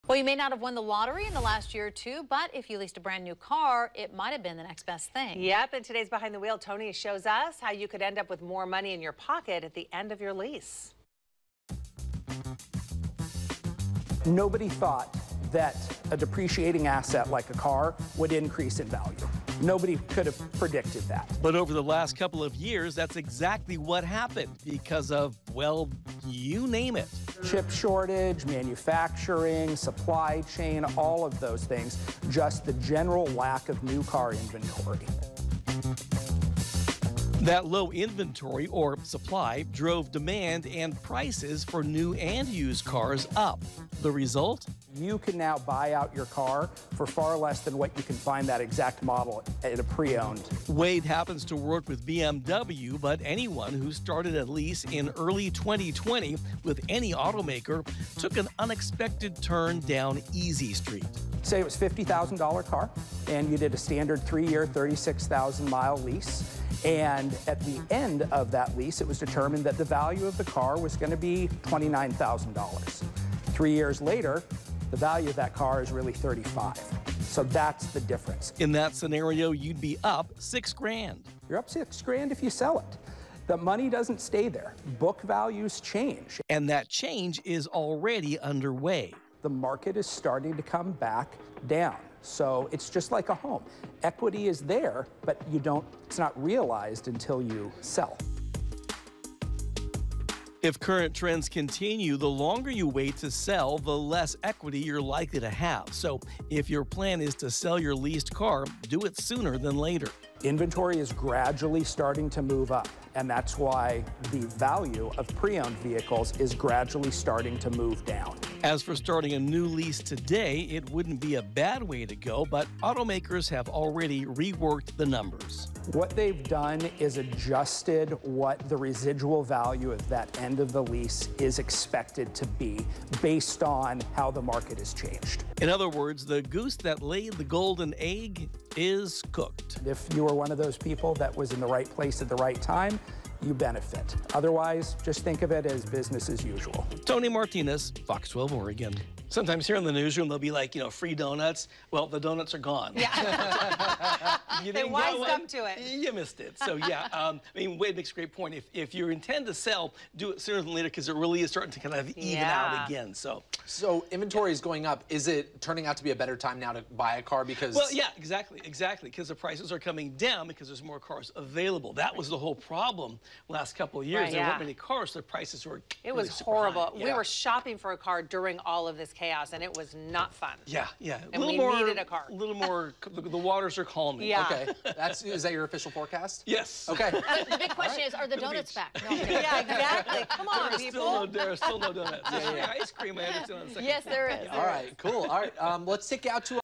Behind the Wheel: People who leased cars in 2020 can buy for a profit
If you leased a car in 2020, you may be in luck. In this Behind the Wheel, Tony Martinez will explain why you may have hit the jackpot and how you can cash in.